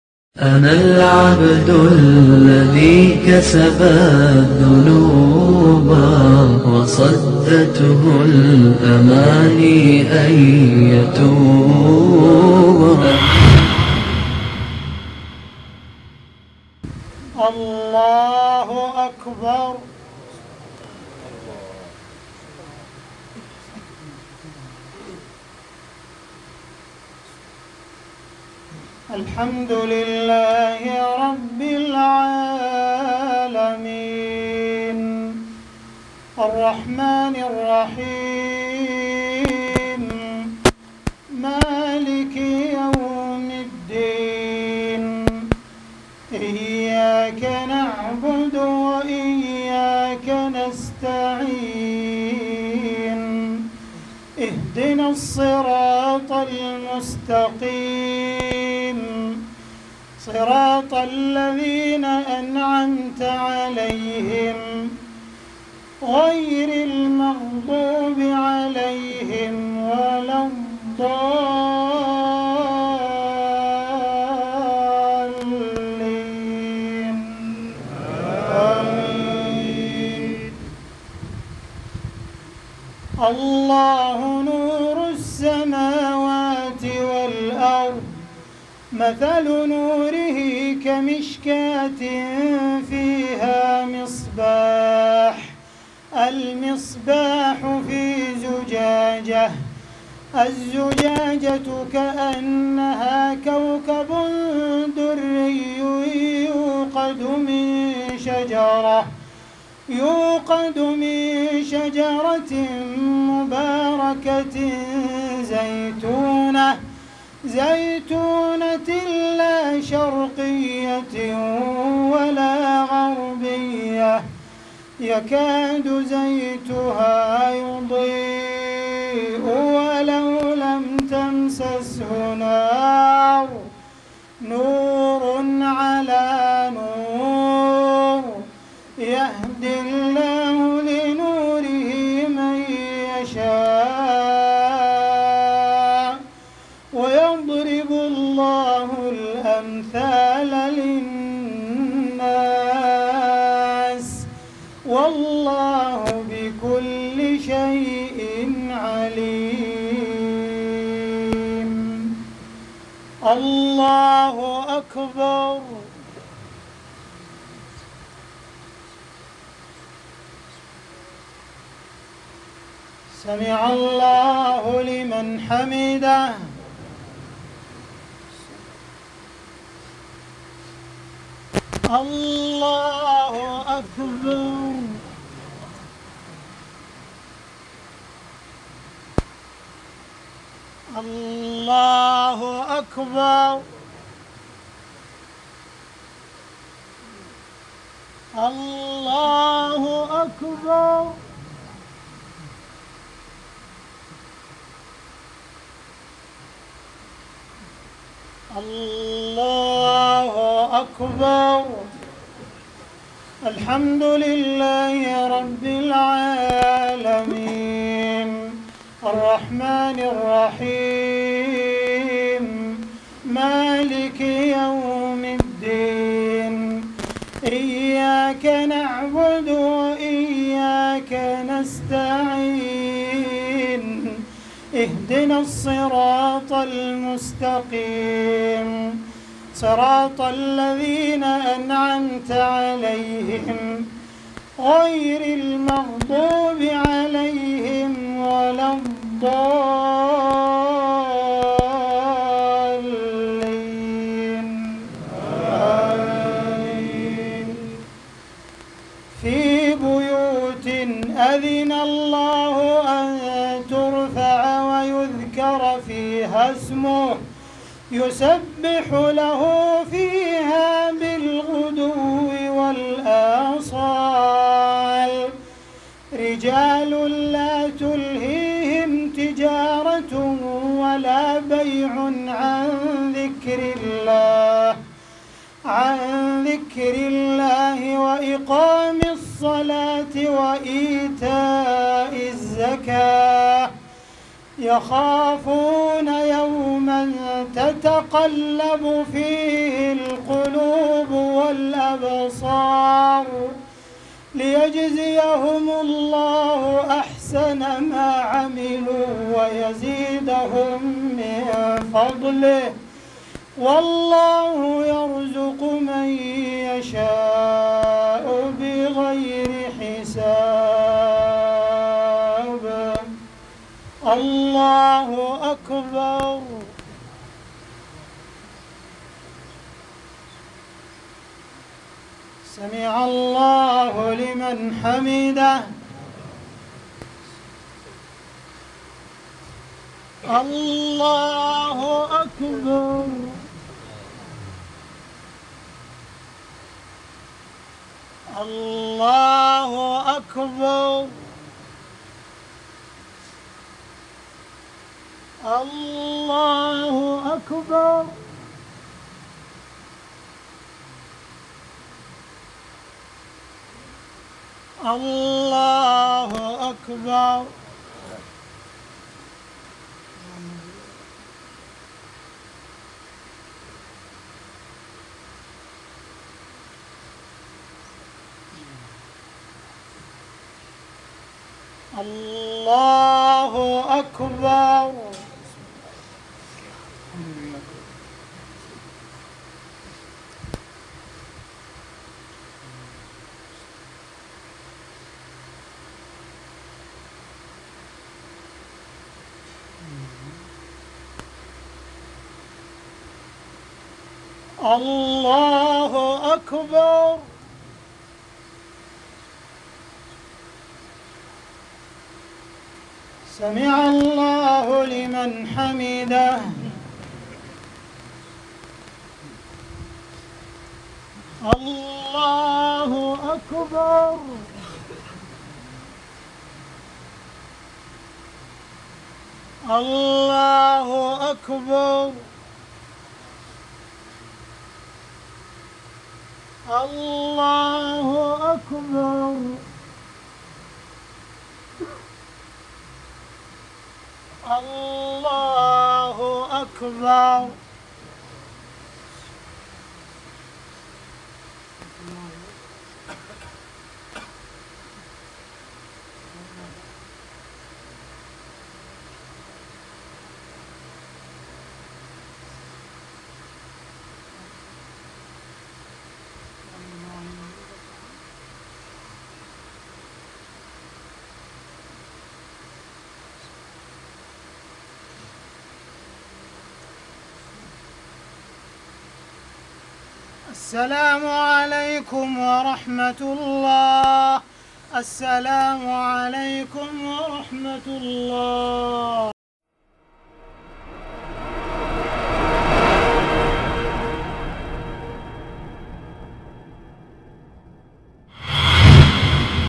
Sheikh Abdul Rahman Al-Sudais Leading Maghrib Prayer in masjid e Ali Birmingham UK (20 july 2016)
تلاوت نماز مغرب
الشیخ عبدالرحمن السدیس امام مسجد الحرام مکۃ المکرمہ